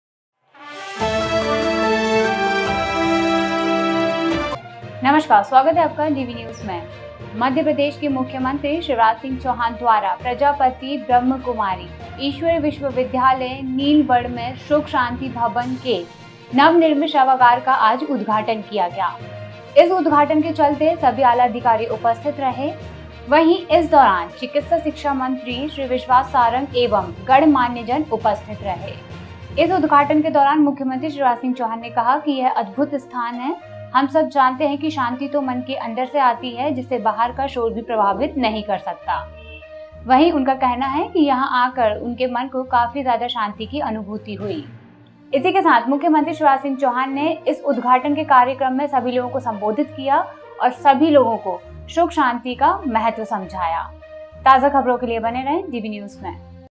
CM ने दीप प्रज्ज्वलन कर किया "अनुभूति सभागार" का उद्घाटन | DV News Live
नर्मदापुरम और देश-दुनिया की अन्य बड़ी ख़बरें पाने के लिए DV News Live से जुड़े रहें. नर्मदापुरम जिले की सभी ख़बरों से जुड़े रहने के लिए हमारे सोशल मीडिया अकाउंट फॉलो करें.